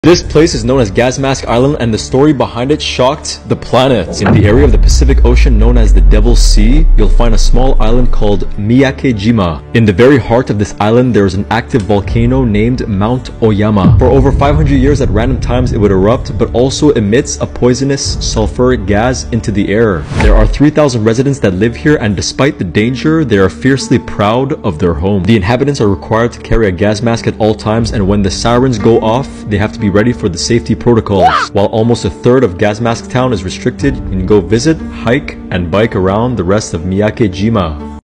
Everyone On This Island Has To Wear A GAS MASK - Miyake Jima..👹
In this video, we explore the story of the town on the Island of Miyake-jima. There’s a volcano called Mount Oyama that can blow. The population wants to stay rather than leave. Everyone needs to wear a gas mask.😧